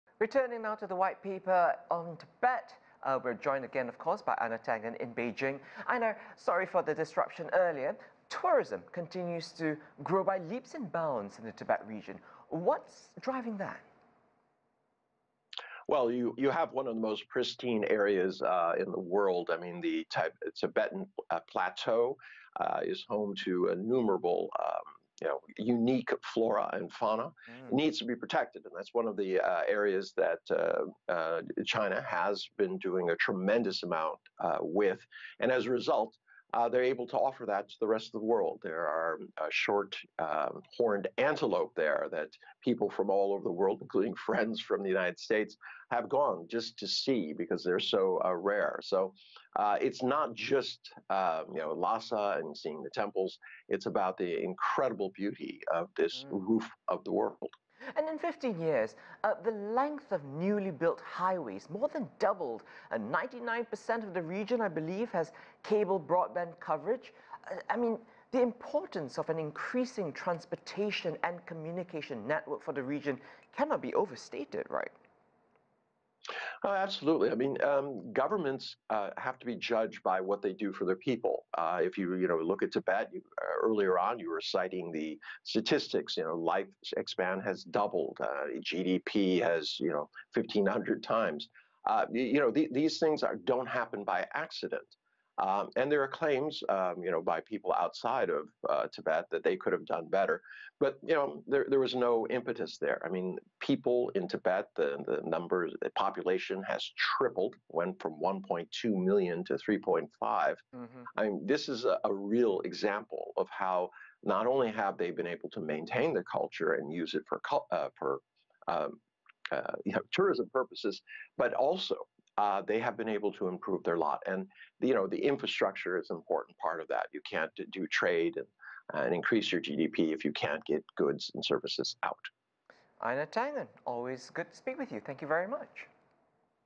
Einar Tangen on China issues white paper on Tibet's peaceful liberation, achievements
CGTN's Gerald Tan talks to political and economic affairs commentator Einar Tangen about China issues white paper on Tibet's peaceful liberation and achievements.